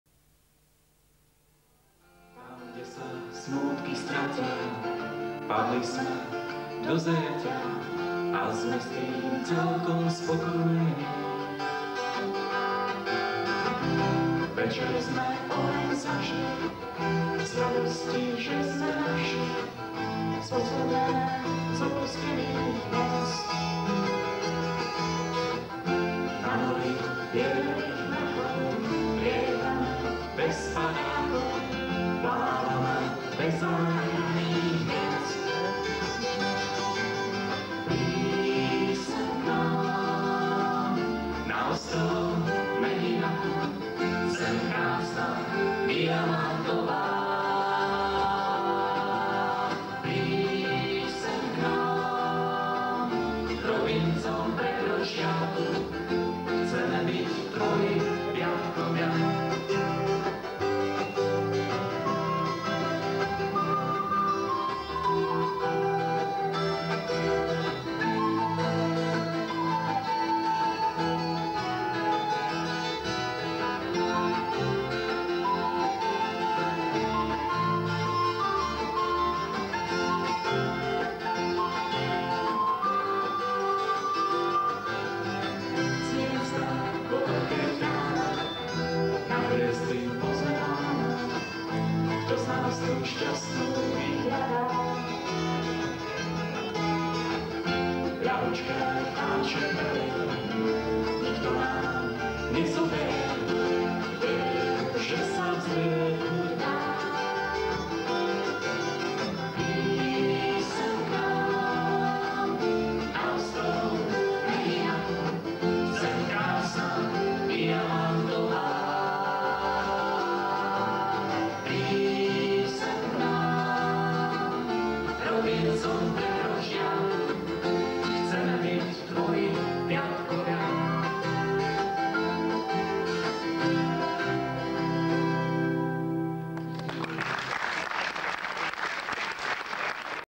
Rolničky COUNTRY 1994
záznam z VHS - rok 1994 - z vystúpenia skupiny Rolničky na festivale COUNTRY 94 na Vajnorskej v Bratislave